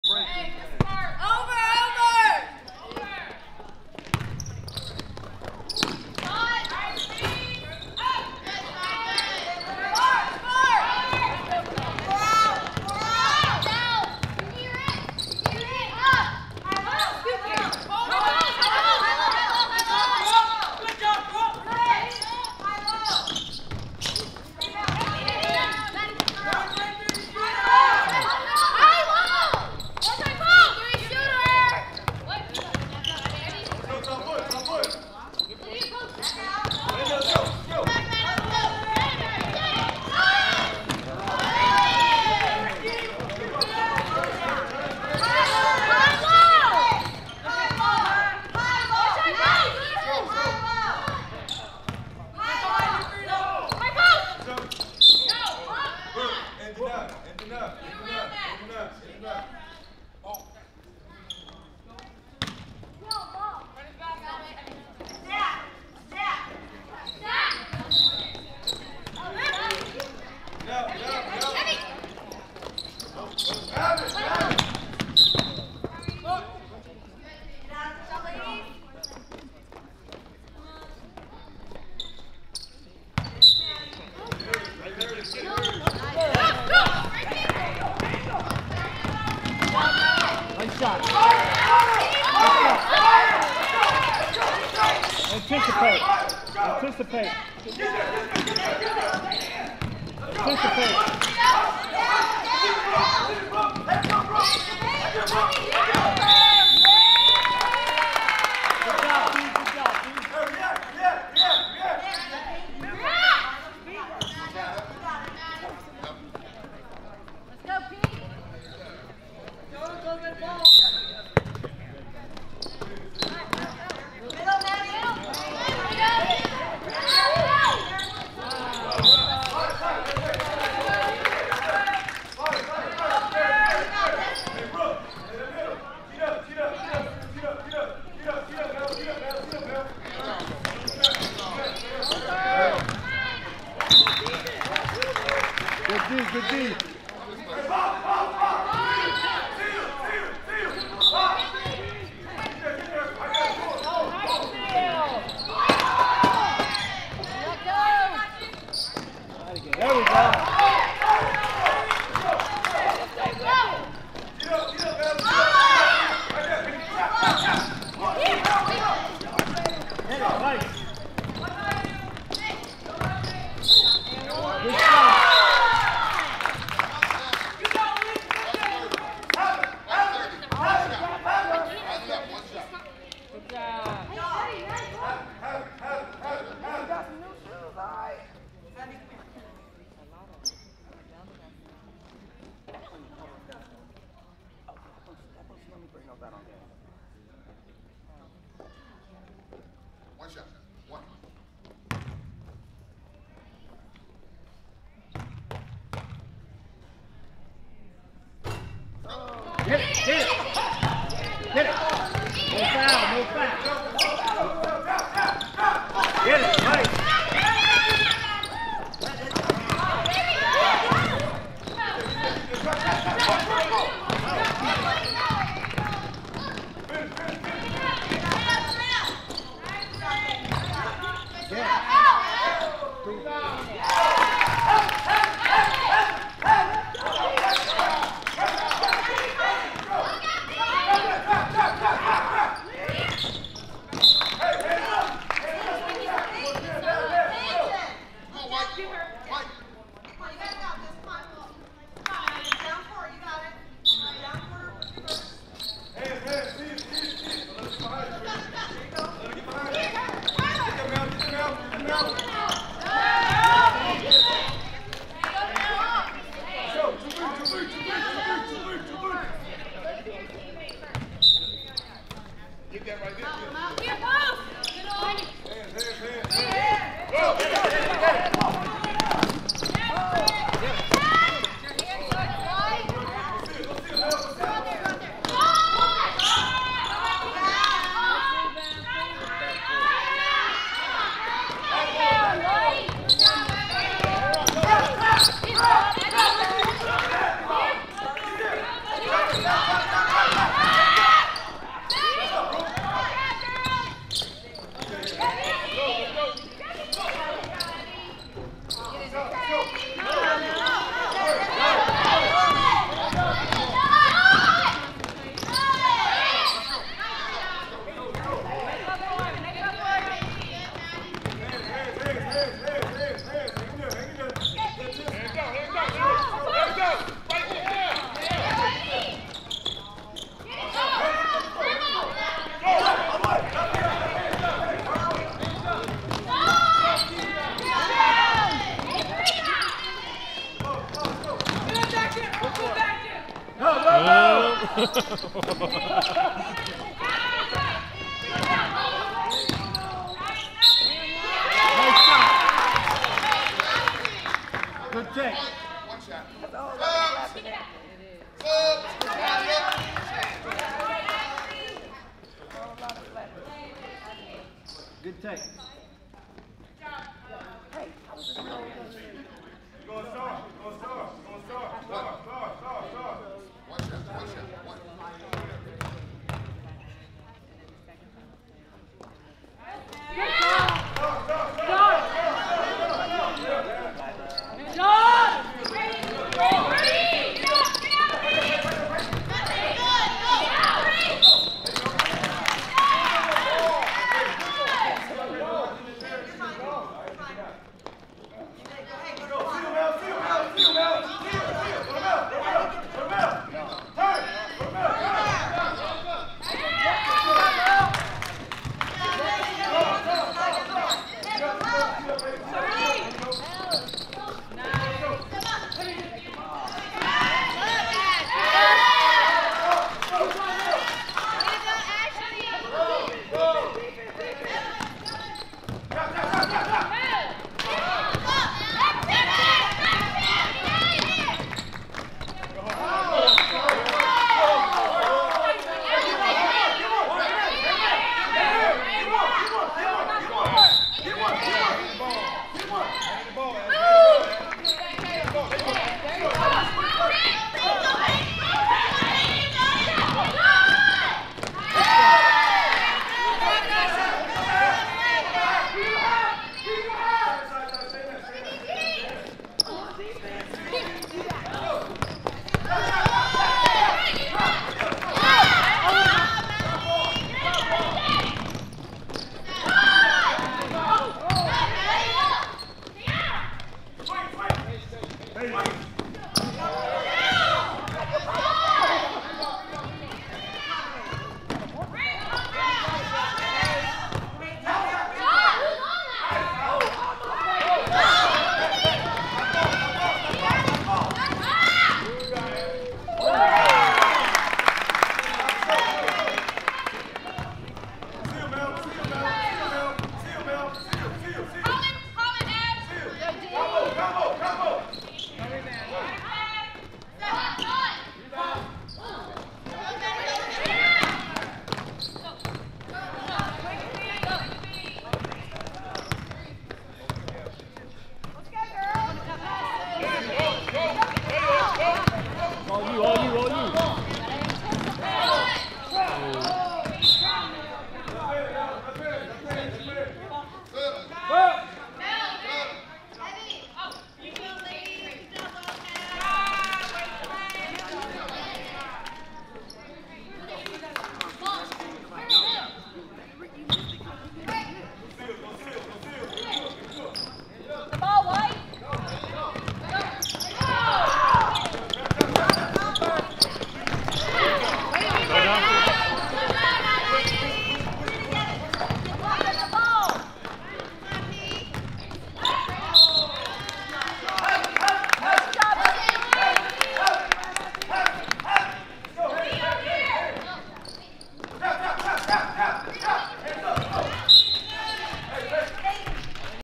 TDBA Lady Wildcats (Kaylee) vs Peak Performance ATL | H1 P1
13th Annual Only the Best Classic
6th-grade girls team A
Tony Delk Basketball Academy